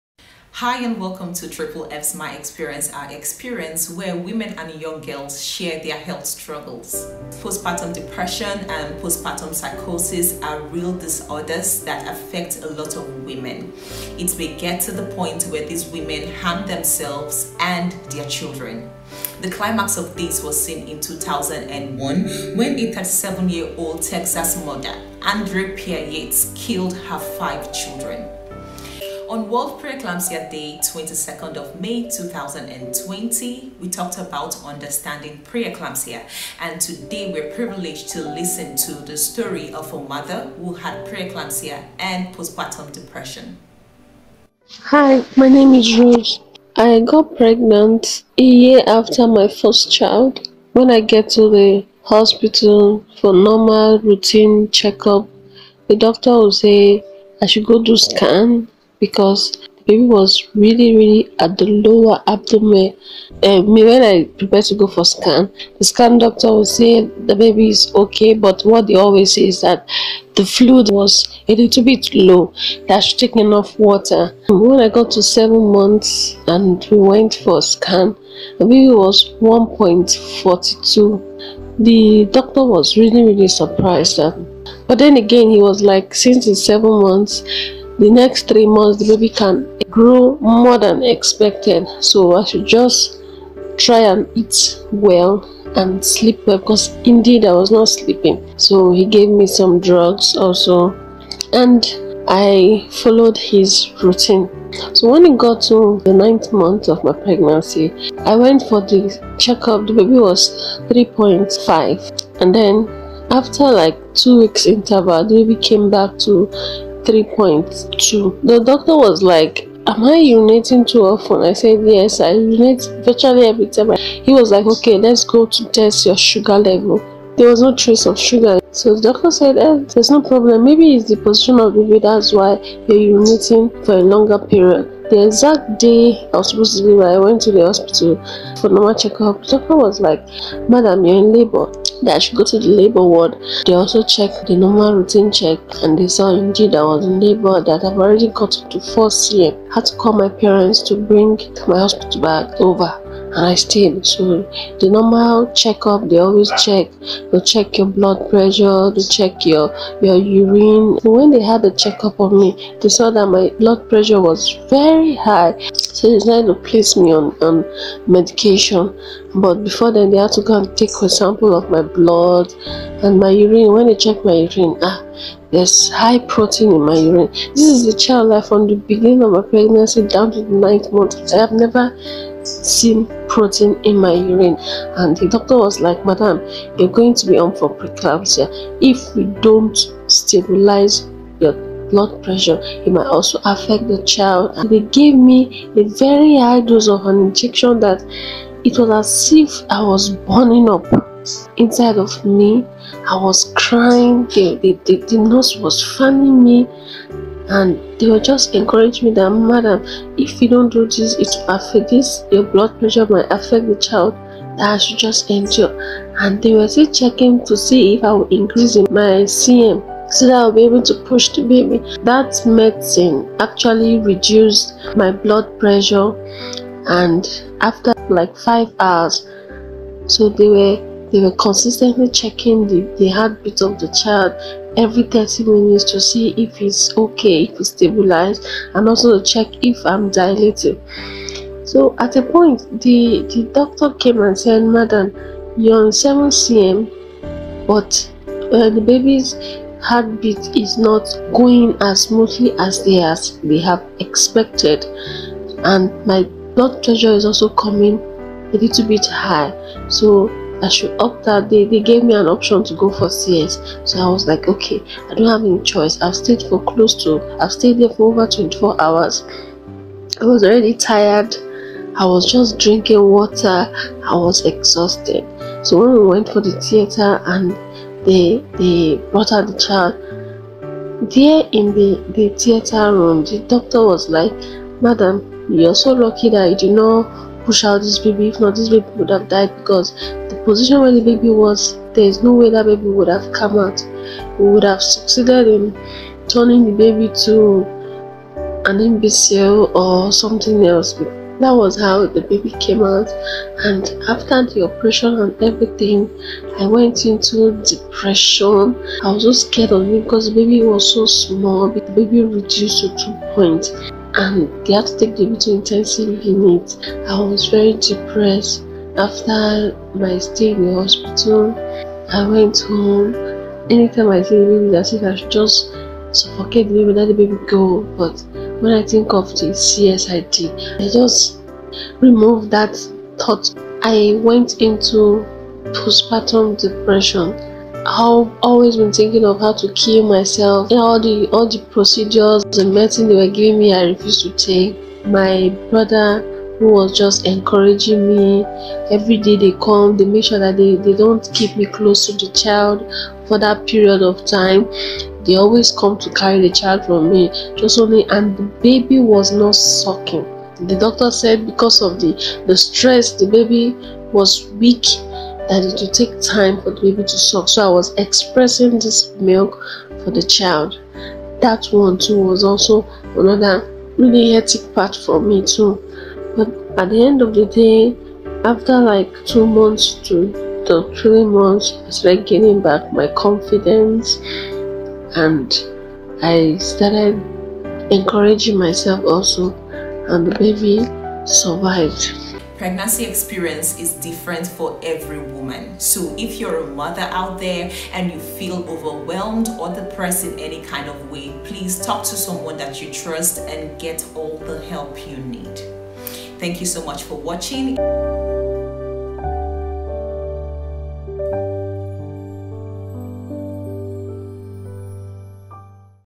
"I thought of k1lling my baby" - Triple F's My Experience Her Experience
As we end the year with the last episode of the series My Experience Her Experience, we encourage more women to speak up to trusted family and friends. On this episode, Rose was kind enough to share her pregnancy experience that led her into postpartum depression with thoughts of harming her baby.

We appreciate all the women that came forth to share their stories publicly. The number of lives you have touched remain tremendous. Thank you for speaking up, thank you for seeking help and we thank God for our restoration despite our traumas.

May 2024 bring to us all beautiful memories and experiences and may we be blessed with wisdom to raise our children right.